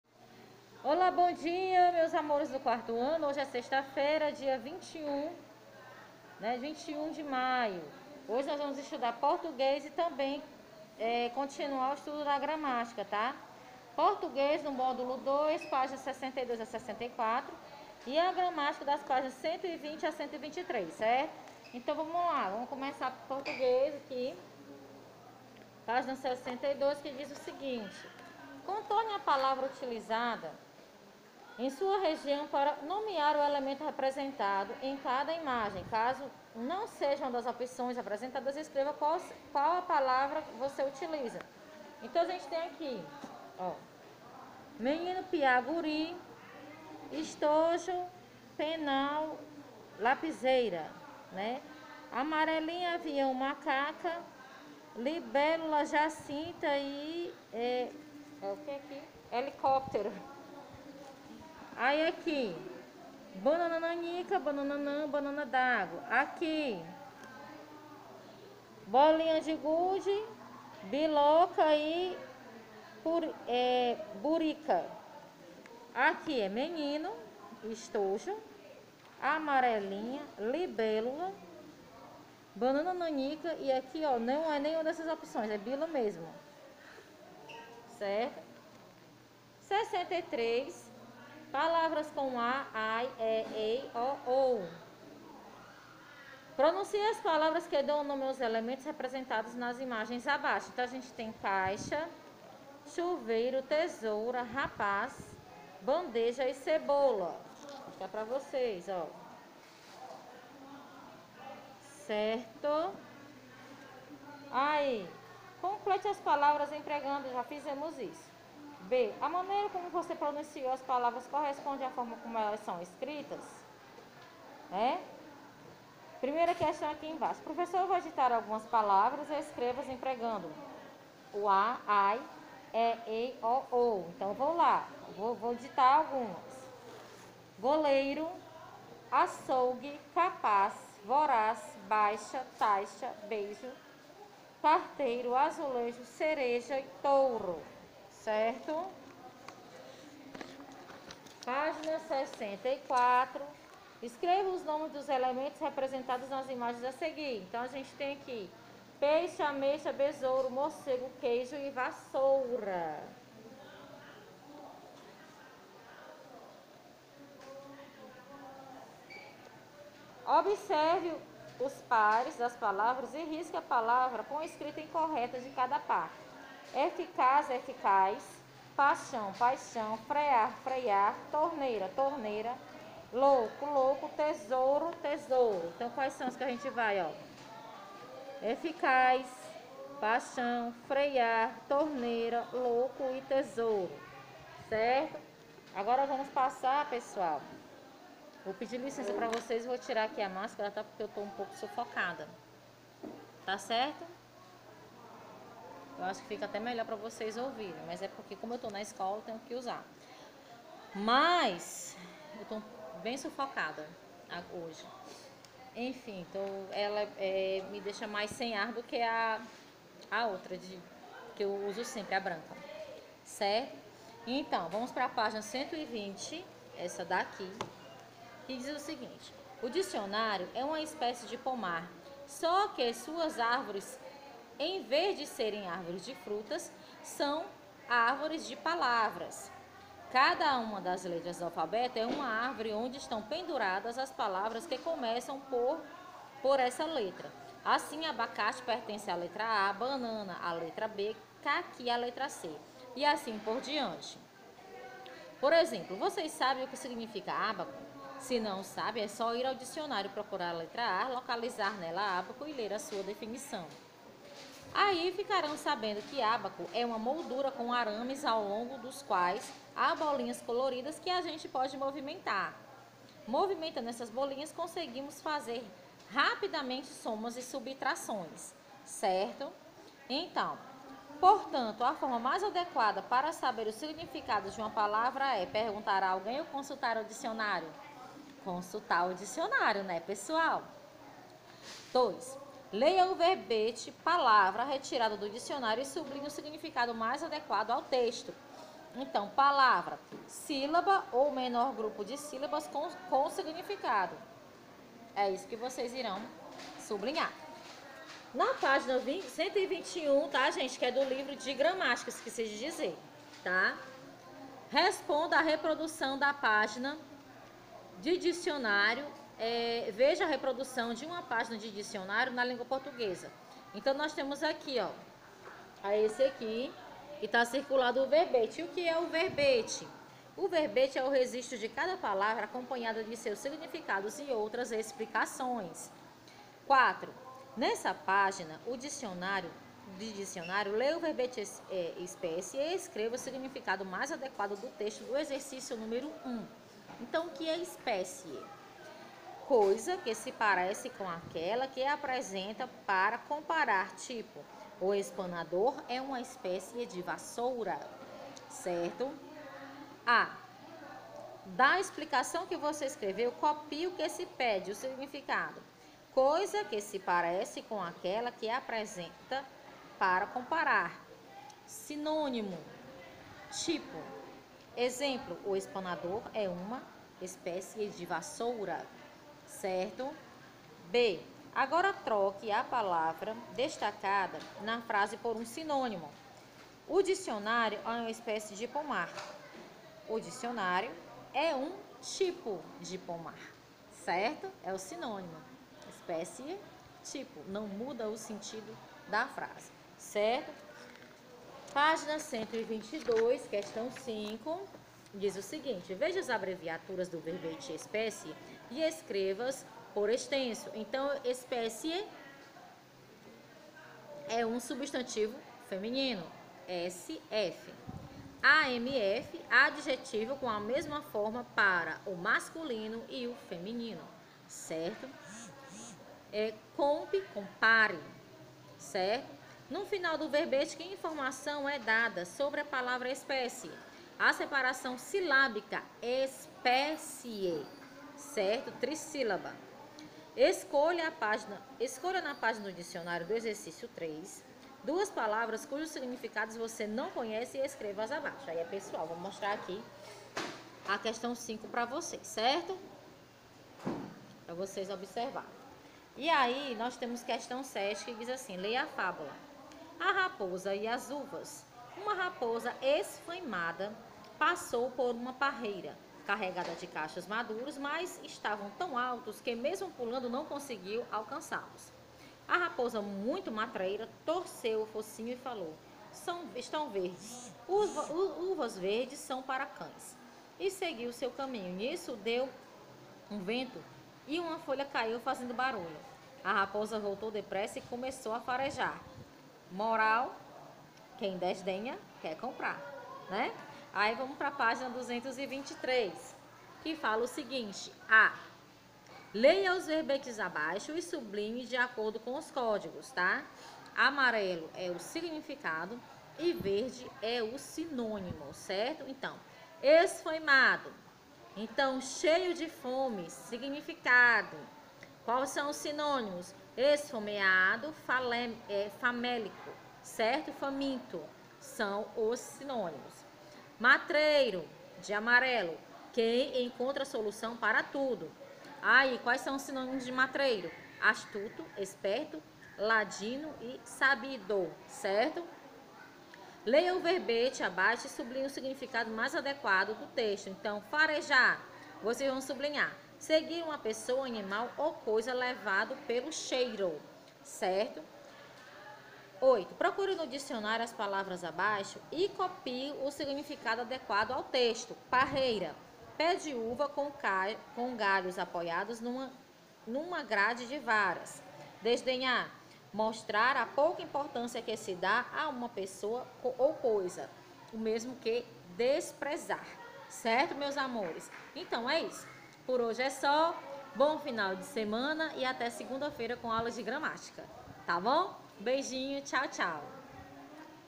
21/05 - 4º Ano Aula de Português
Português: Mód. 02, Cap. 6 - Palavras com a /ai/e/ei/ o/ ou páginas: 62 a 64.
Gramática: Ortografia Uso do dicionário 2 páginas 120 a 123.